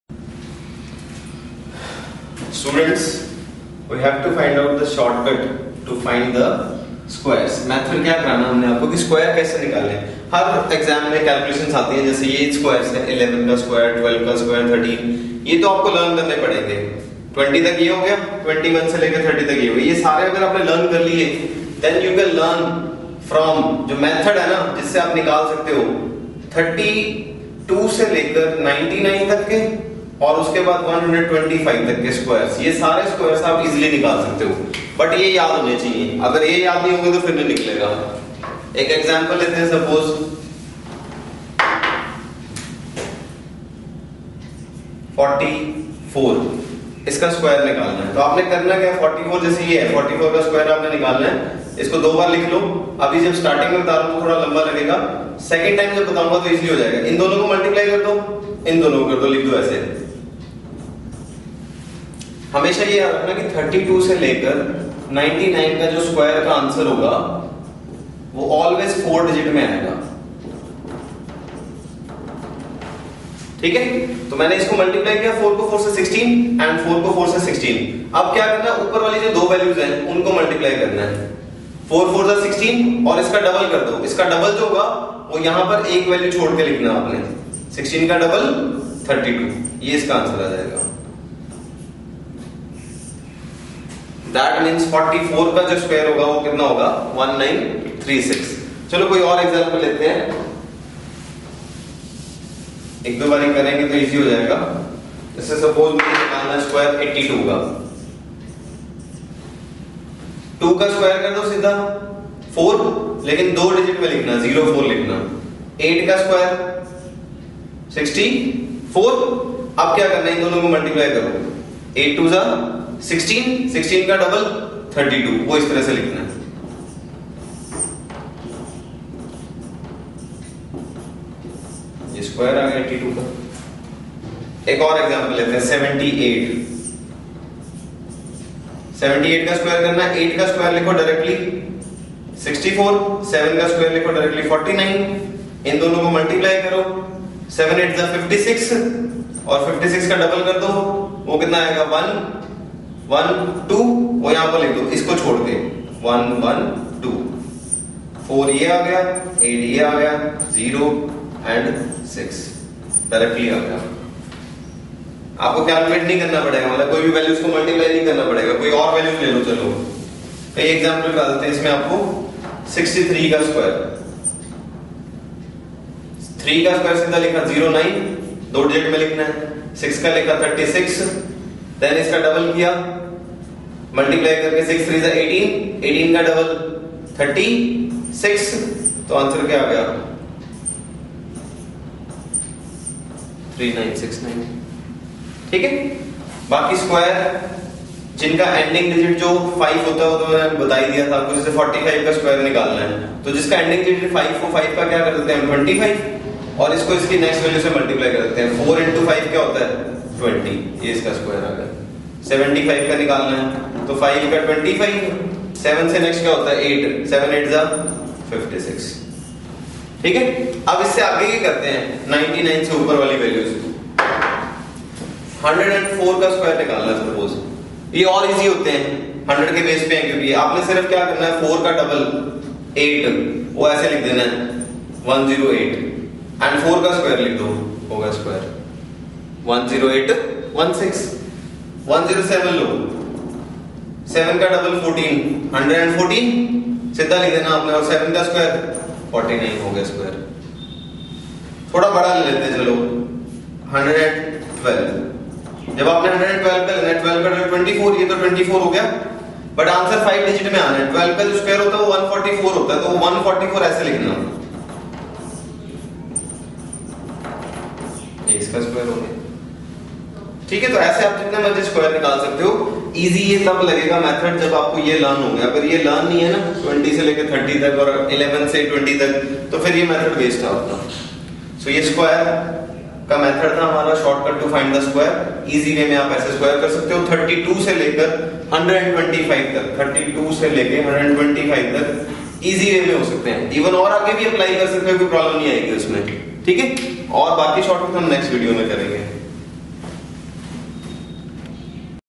maths Shortcut to find square of any number-Maths Reasoning Aptitude Tricks for Competitive exams
प्रतियोगी गणित के सभी अध्याय - All Math Tricks in Hindi (Chapters/Quiz/Downloads) | Hindi Audio Notes